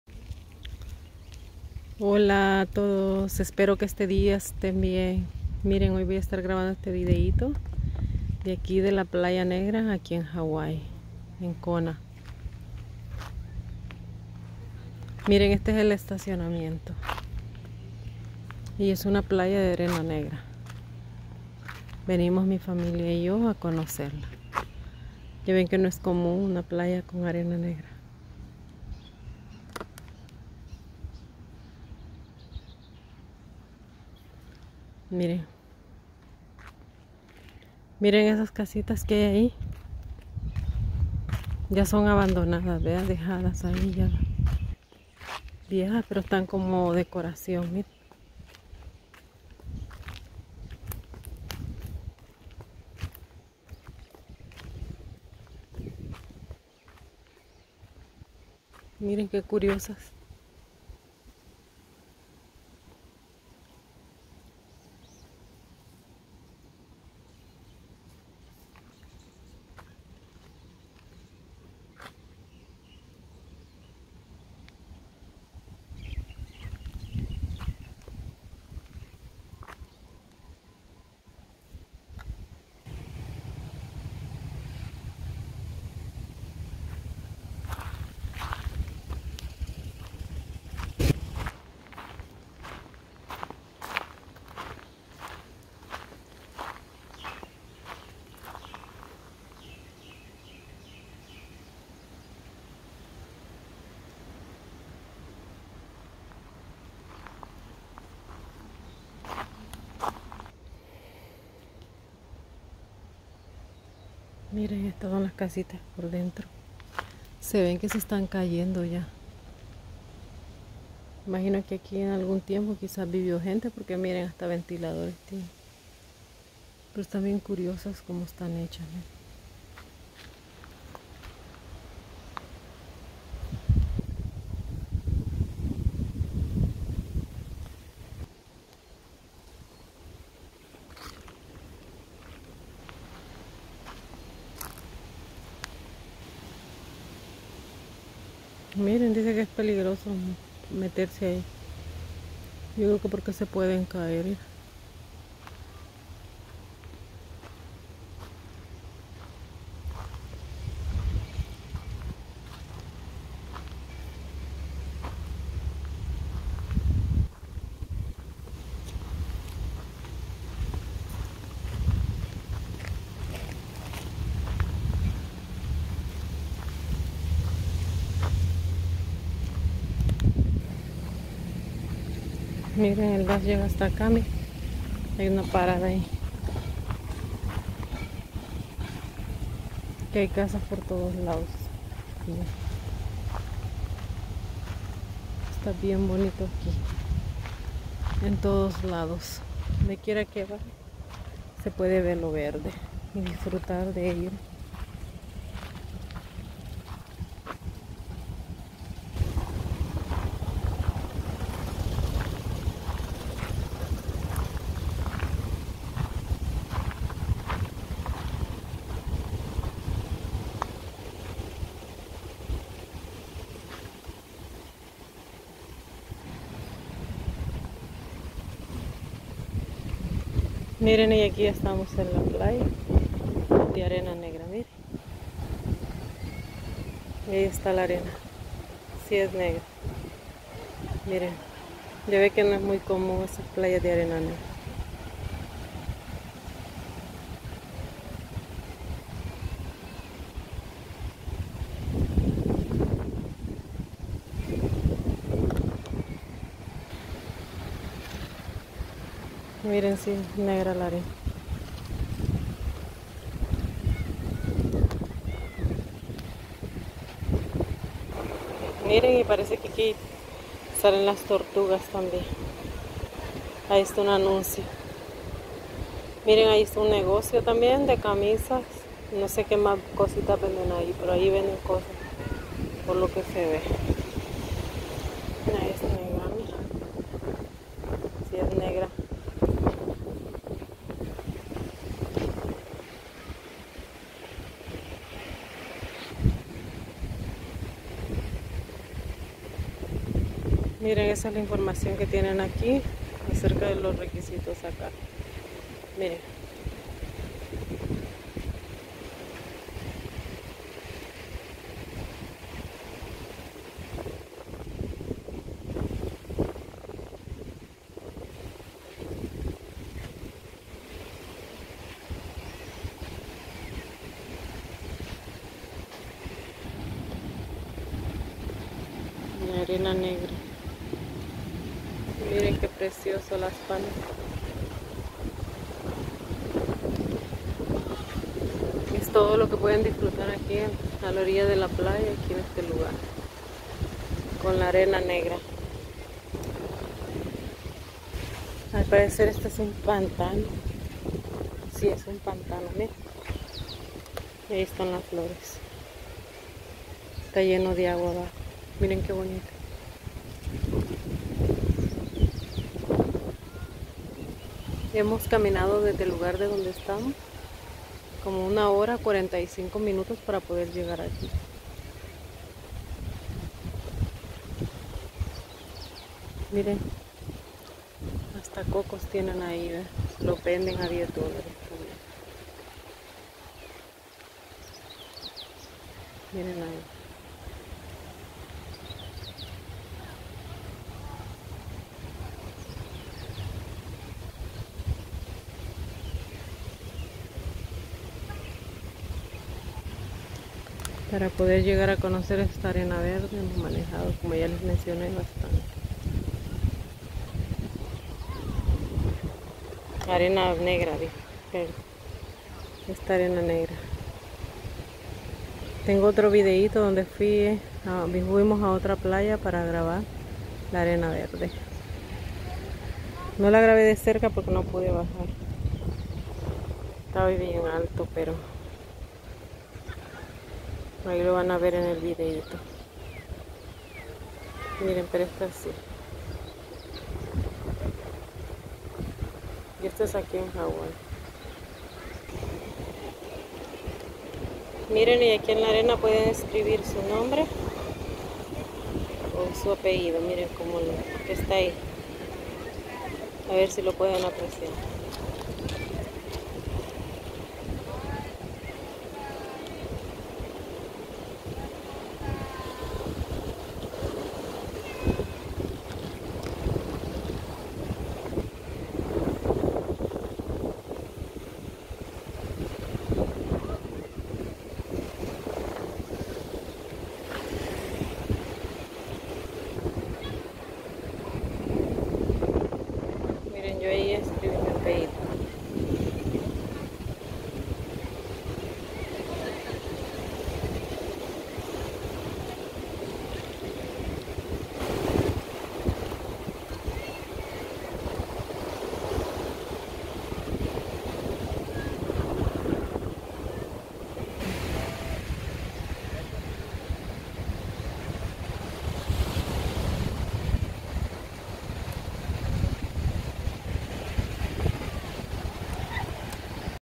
Playa de arena negra en la isla de Kona Hawaii.
Este lugar se encuentra en playa de arena negra de Punalu'u en la isla grande de Kona Hawaii. La entrada es gratuita también pueden ver tortugas marinas y hay muchos lugares para visitar cerca de aquí,como él parqué nacional de los volcanes.